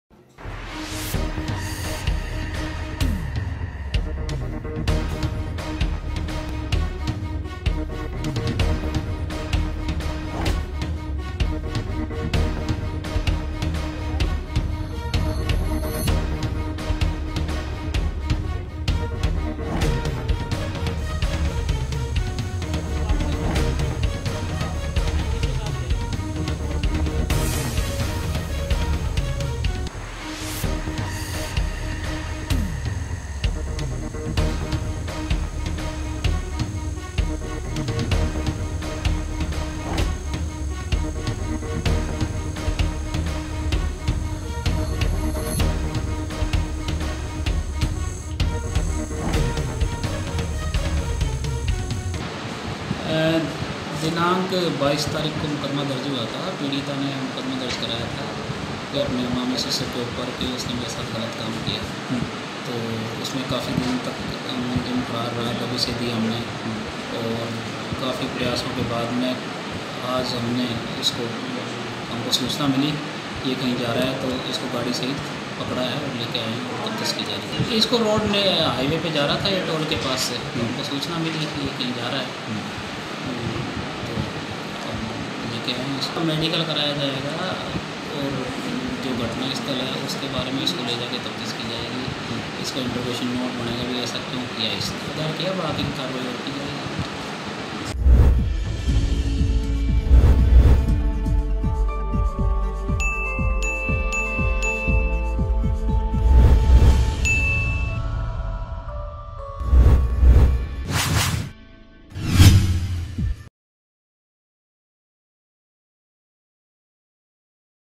Kishangarh NEWS | रिश्तो को शर्मसार करने वाली घटना | MTTV INDIA
रिश्तो को शर्मसार करने वाली घटना
मदनगंज थाना ने पकड़ा दुष्कर्म का आरोपी
भांजे की पत्नी के साथ दुष्कर्म का मामला
आरोपी मामा ससुर सत्यप्रकाश माहेश्वरी पुलिस गिरफ्त में
जीवीके टोल के पास से कार सहित आरोपी को किया गिरफ्तार
मदनगंज थाना पुलिस जुटी मामले की जांच में